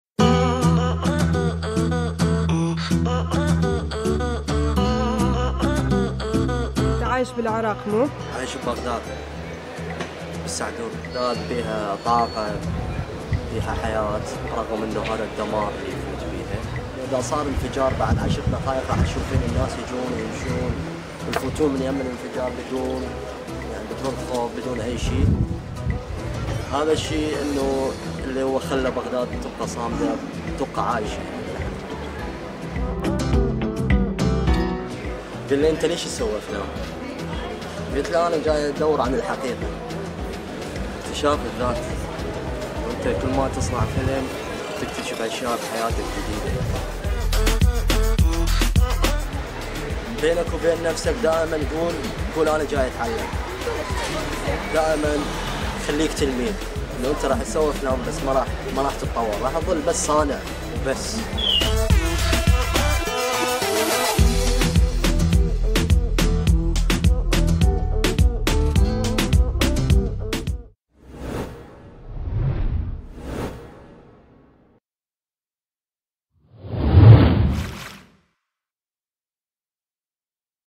لقاء مع مخرج عراقي
يجيب المخرج العراقي علي رحيم على أسئلتنا: "بغداد مليئة بالحياة"!

Wada7 Original